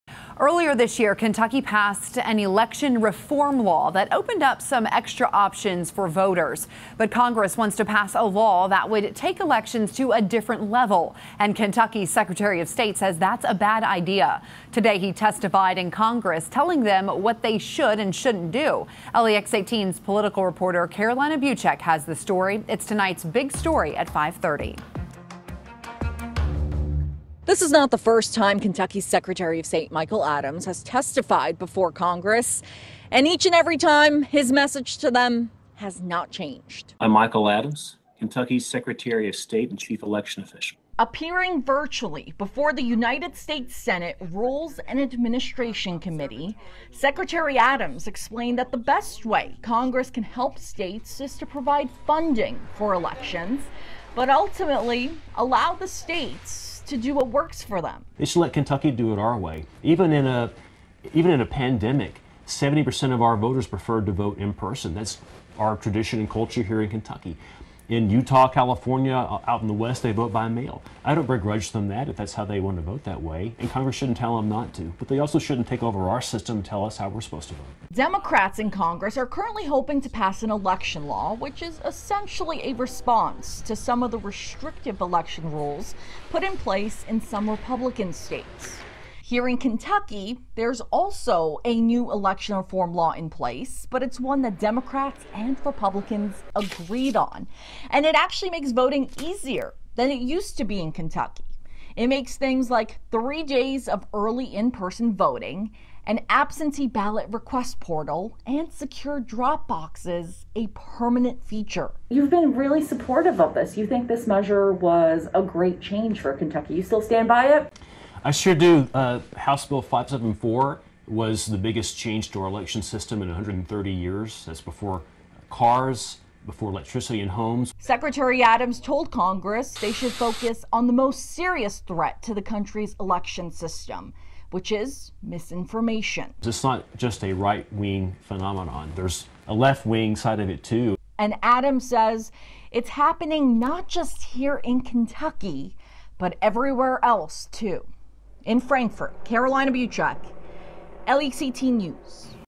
Secretary of State: Stay out of Kentucky elections
Kentucky's Secretary of State, Michael Adams testified today in Congress pertaining to Kentucky elections.